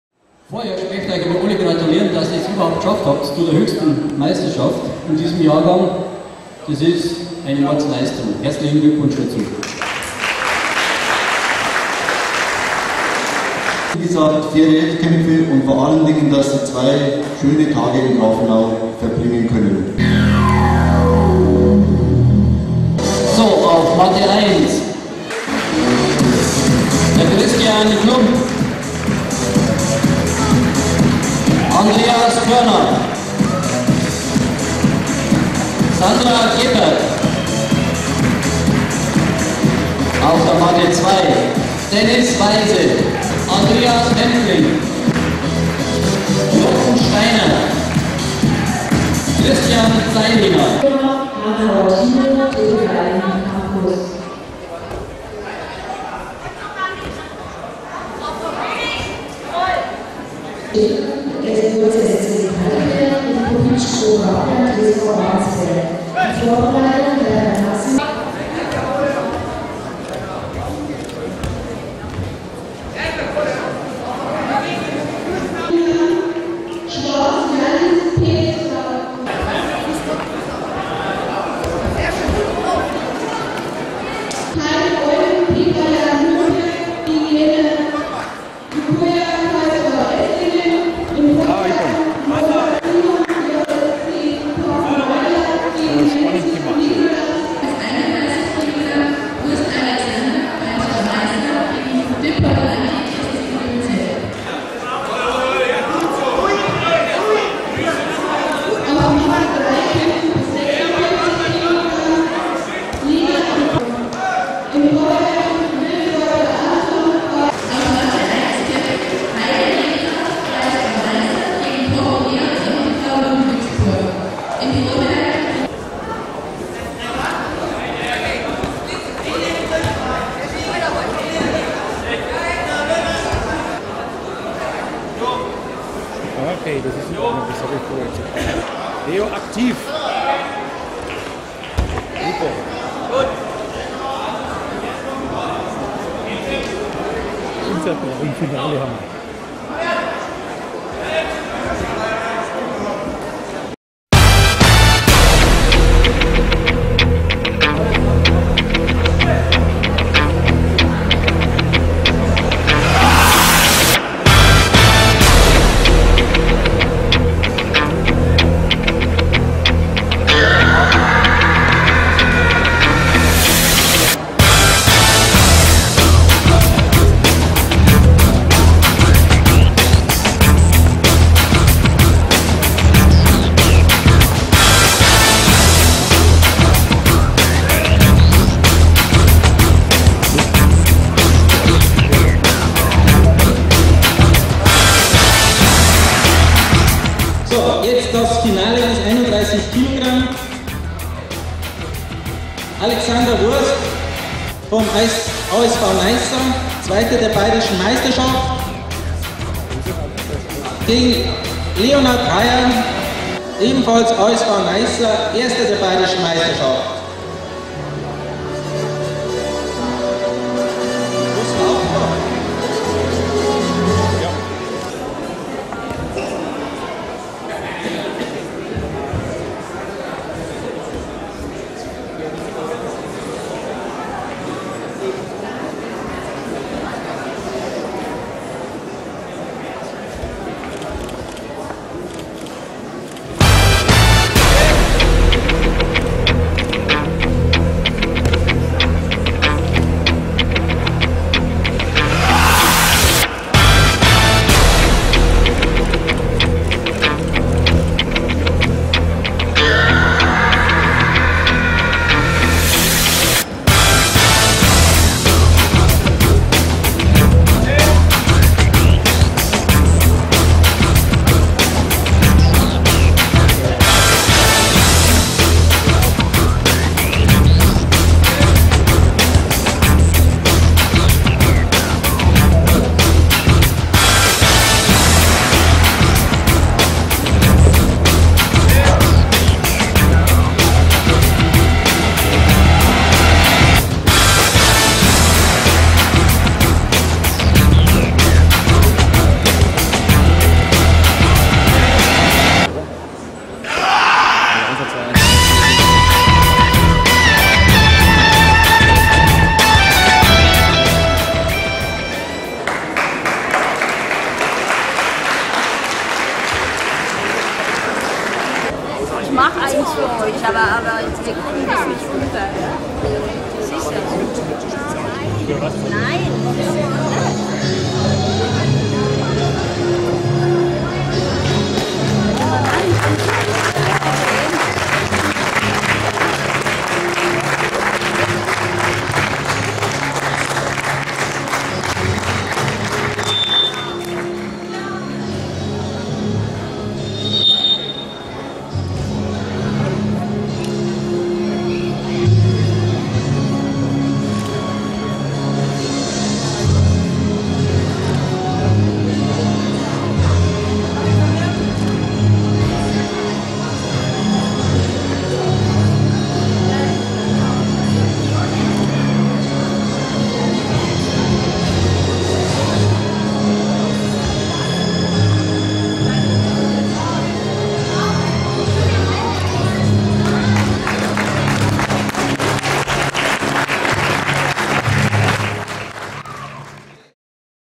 Süddeutsche EM MU14 in Grafenau am 20.10.12
Ausschnitte der Kämpfe der Teilnehmer des ASV Naisa Ellerntal, Leo Heyder und Alex Wurst, die sich im Finale der Gewichtsklasse -31 kg gegenüberstanden